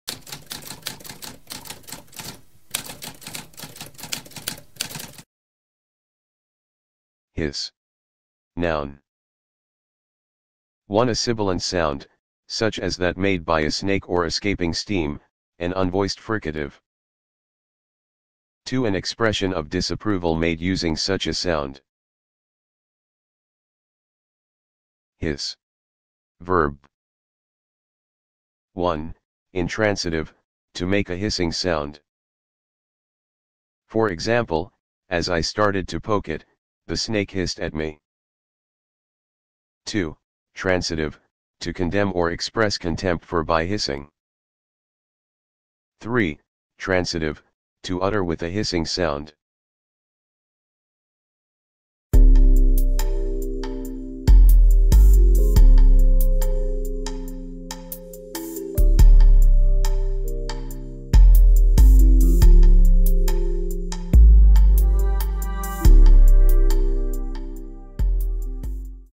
What does hiss mean?
A spoken definition of hiss.

Intro Sound:
Typewriter - Tamskp